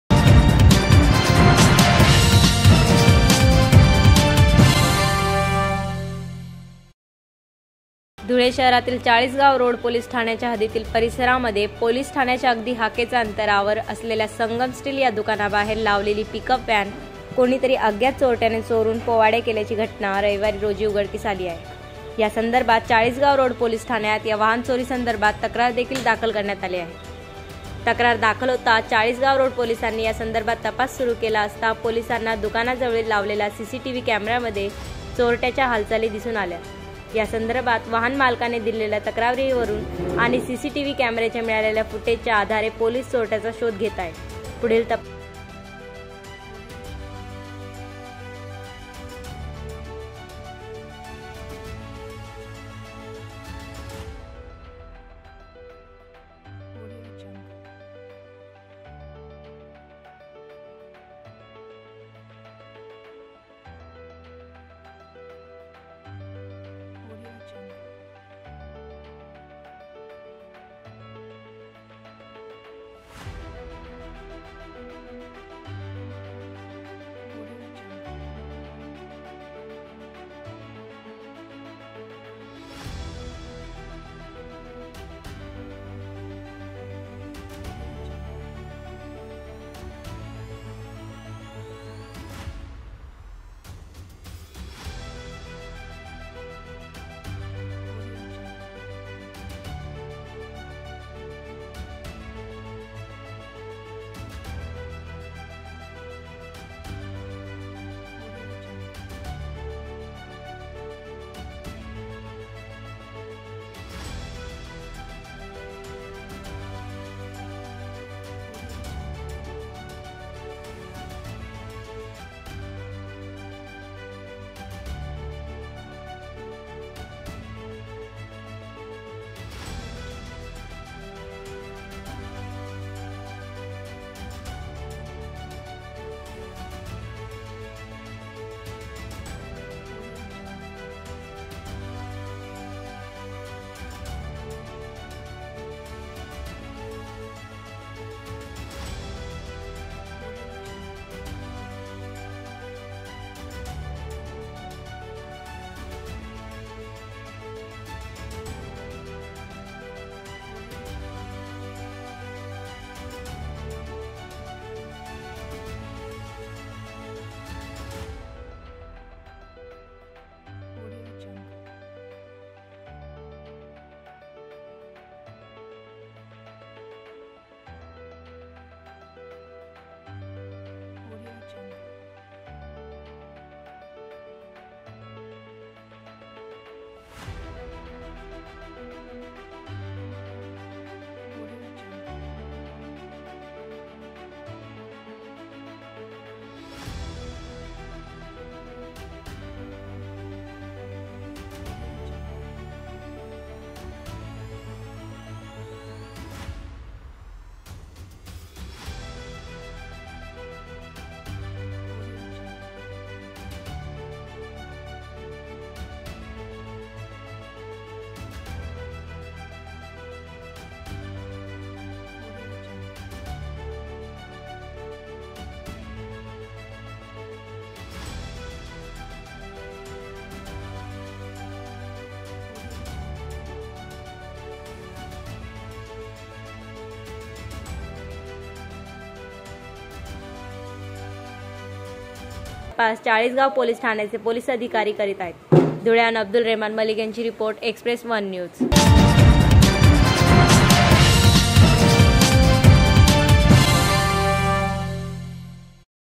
पोलीस ठाण्याच्या हाकेच्या अंतरावरून पिकअप व्हॅन लंपास; सीसीटीव्हीत चोरीच्या हालचाली कैद
रविवारी 17 जुलै रोजी ही चोरी उघडकीस आल्याने परिसरात एकच खळबळ उडाली आहे.

धुळे शहरातील चाळीसगाव रोड पोलीस ठाण्याच्या हद्दीतील परिसरामध्ये पोलीस ठाण्याच्या अगदी हाकेचा अंतरावर असलेल्या संगमस्टील या दुकानाबाहेर लावलेली पिकअप व्हॅन कोणीतरी अज्ञात चोरट्याने चोरून पोवाडे केल्याची घटना रविवारी रोजी उघडकीस आली आहे.

या संदर्भात चाळीसगाव रोड पोलीस ठाण्यात या वाहन चोरी संदर्भात तक्रार देखील दाखल करण्यात आले आहे.

तक्रार दाखल होताच चाळीसगाव रोड पोलिसांनी या संदर्भात तपास सुरू केला असता, पोलिसांना दुकाना जवळील लावलेल्या सीसीटीव्ही कॅमेरामध्ये चोरट्याच्या हालचाली दिसून आल्या आहेत, या संदर्भात वाहन मालकाने दिलेल्या तक्रारीवरून आणि सीसीटीव्ही कॅमेराच्या मिळालेल्या फुटेजच्या आधारे पोलीस चोरट्याचा शोध घेत आहेत. पुढील तपास चाळीसगाव पोलीस ठाण्याचे पोलीस अधिकारी करीत आहे.

धुळ्याहून अब्दुल रहमान मलिक यांची रिपोर्ट